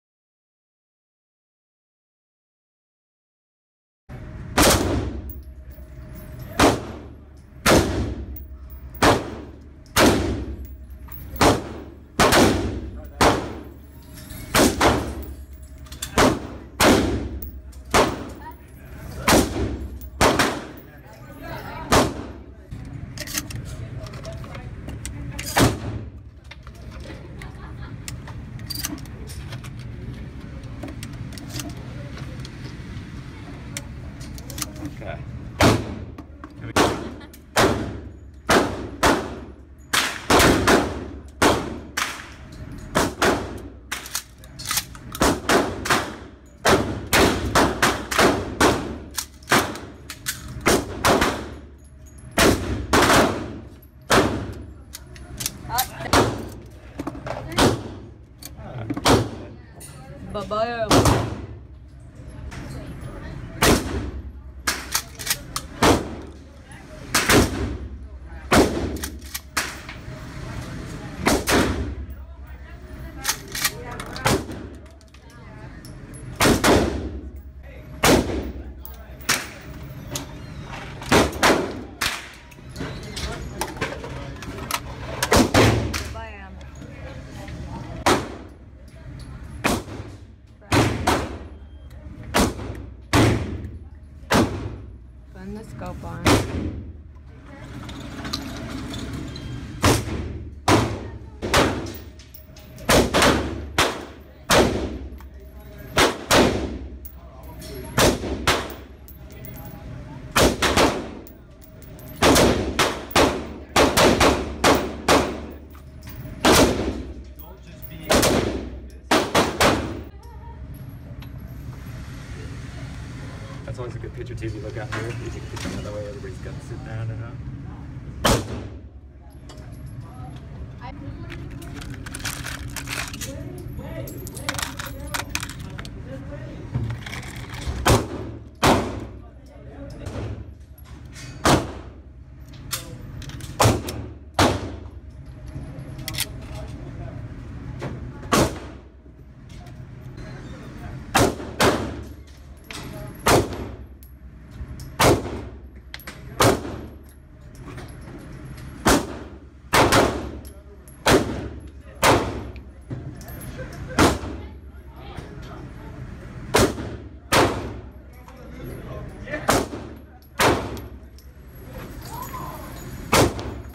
Shooting Range
With The Amanda Jean at the shooting range.